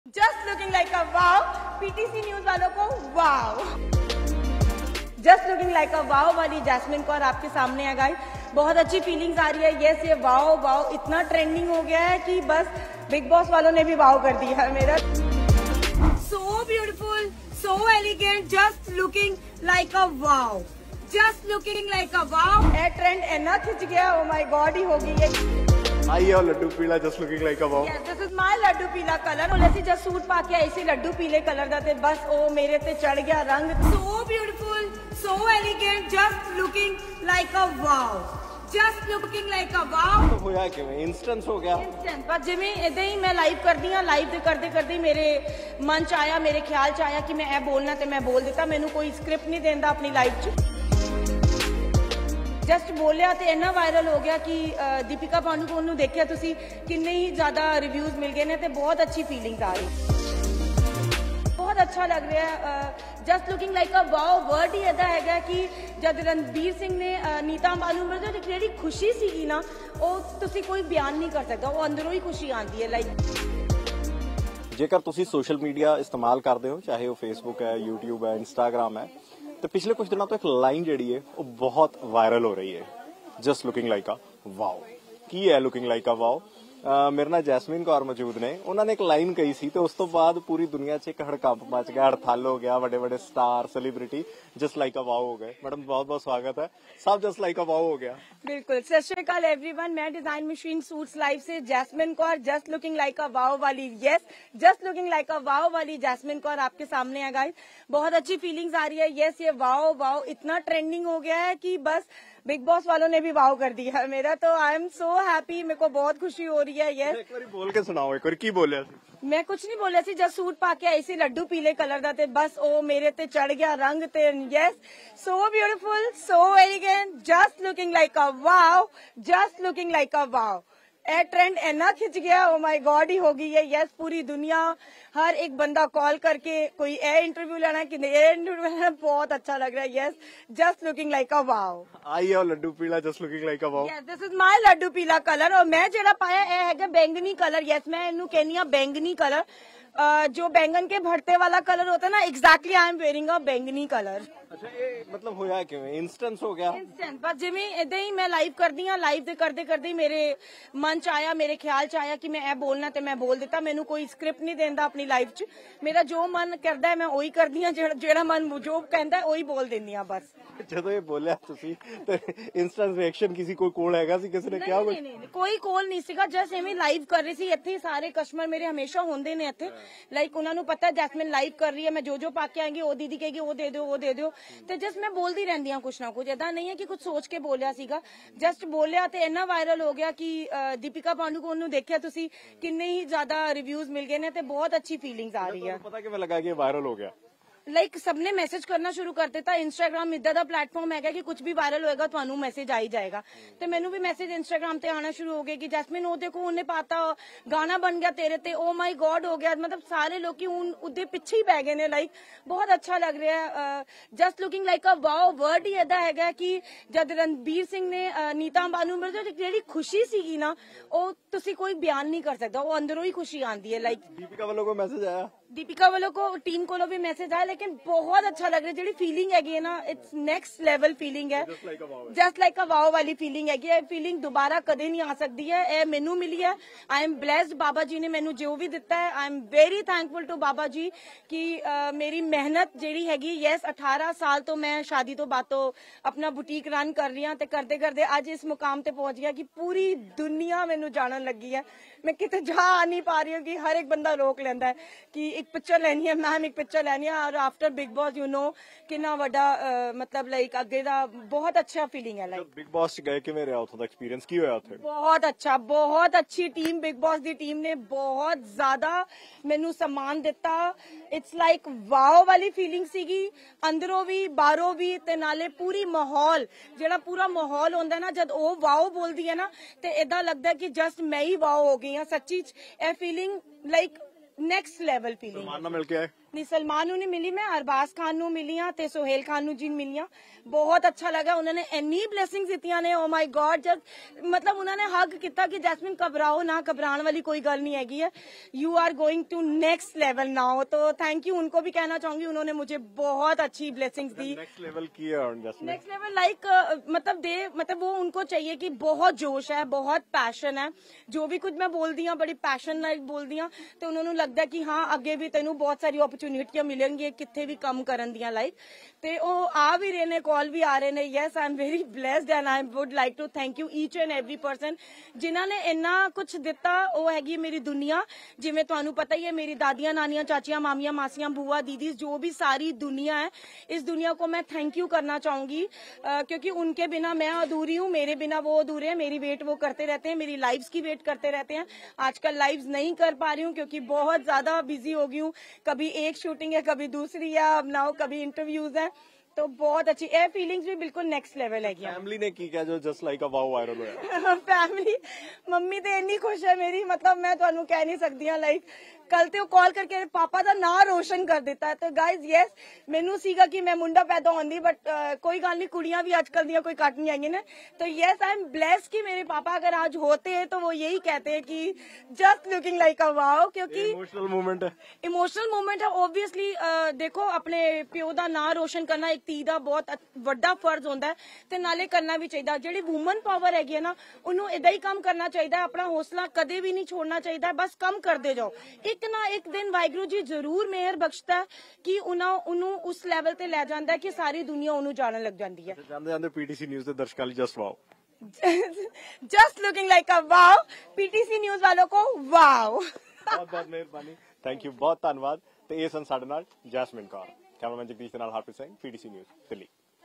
Jasmeen Kaur Interview | Just Looking Like a Wow ਵਾਲੀ ਜਸਮੀਨ ਦੀ Bigg Boss 17 'ਚ ਹੋਈ 'Wow - Wow'
Just Looking Wow ਵਾਲੀ ਜਸਮੀਨ ਕੌਰ PTC News 'ਤੇ .| Bigg Boss 17 ਸ਼ੋਅ 'ਚ ਹੋਈ 'Wow - Wow '